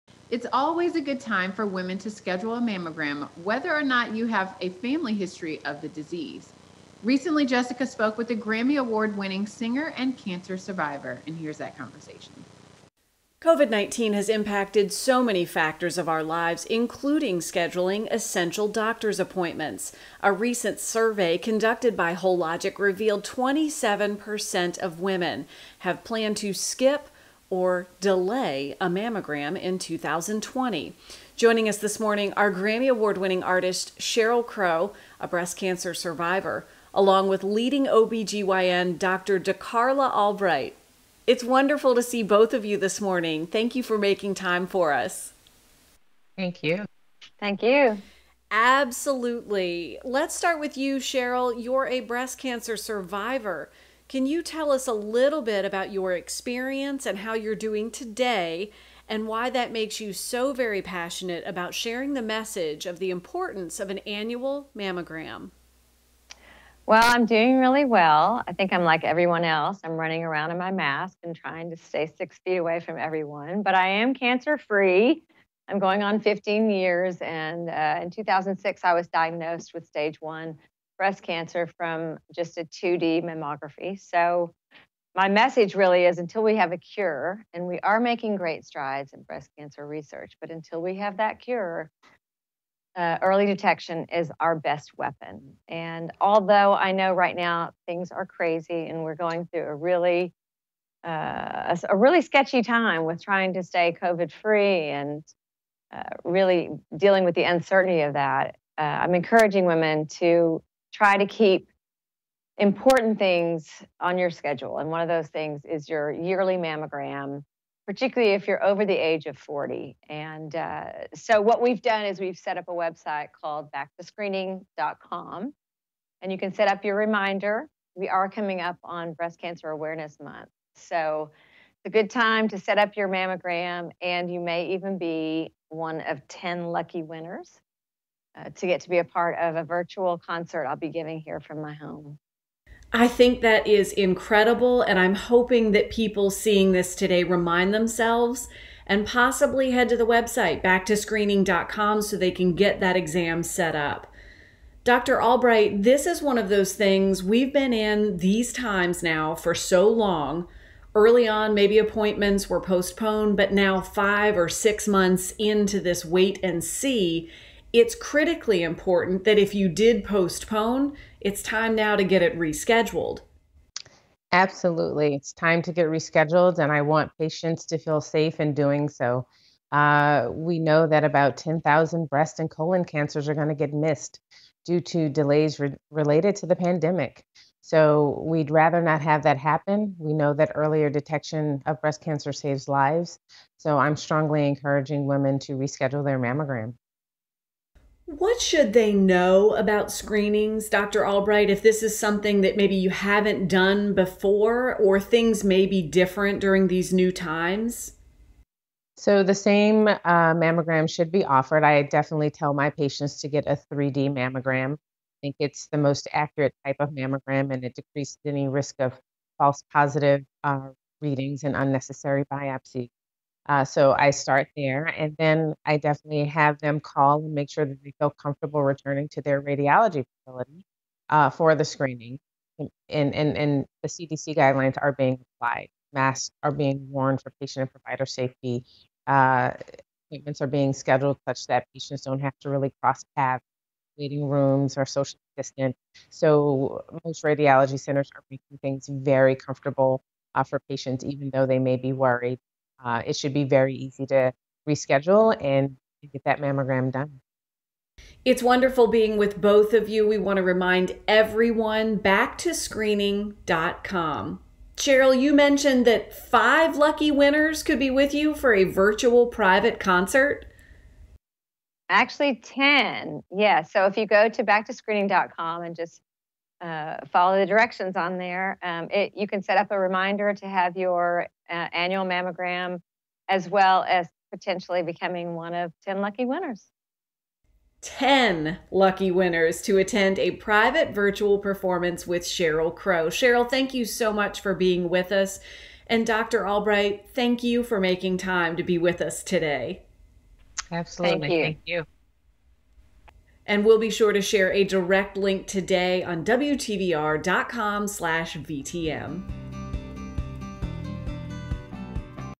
Sheryl Crow shares the importance of mammograms and early detection
Over 15 years ago, Sheryl Crow and her doctors discovered breast cancer on a 2-dimensional mammogram scan, which changed her life. Today, she is cancer free and joins us alongside Dr. Carla Albright, a leading OB/GYN, to encourage women to visit their doctors for screenings.